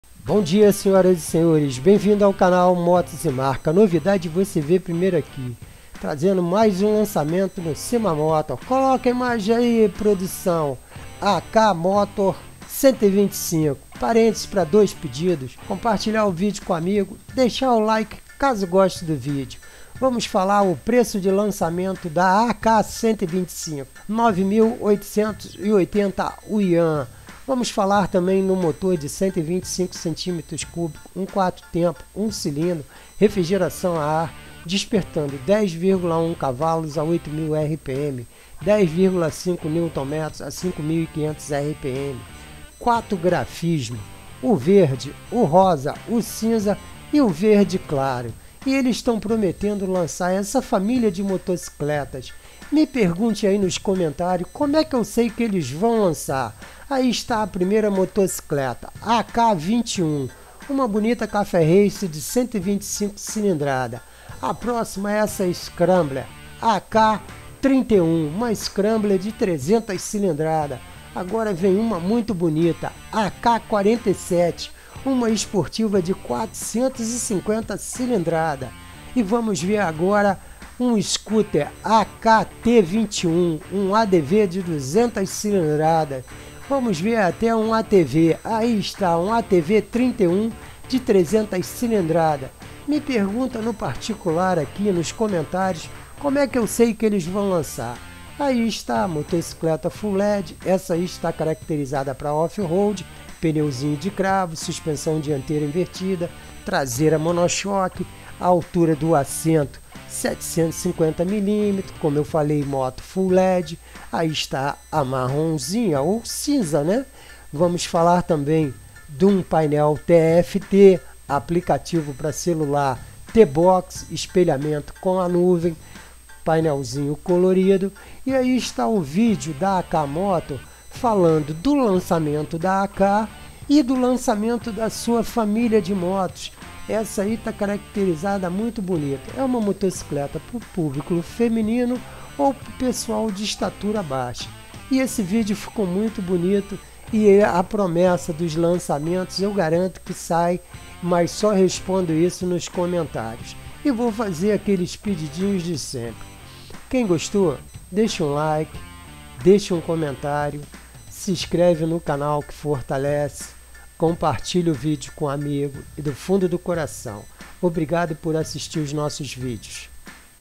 Lançada no CIMAMotor AK MOTOR 125, preço de 9880 yuan, primeiro canal no Brasil a mostra
O vídeo mostra o lançamento da marca AK MOTOR e do modelo AK125 com motor de 125 CC um 4 tempos, 1 cilindro com refrigeração a ar, despertando 10,1 CV a 8000 rpm e 10,5 Nm a 5500 rpm, luzes em LED/DRL, peso 99 Kg, dividido pela potência temos 9,801 Kg/CV, painel TFT com APP, banco inteiriço altura do banco 750mm, suspensão dianteira invertida, traseira monochock, rodas de liga leve, freios a discos em ambas as rodas de 220 mm, pneu dianteiro 120/70-12 e traseiro 130/70-12, apresentação de futuros lançamentos da família AK.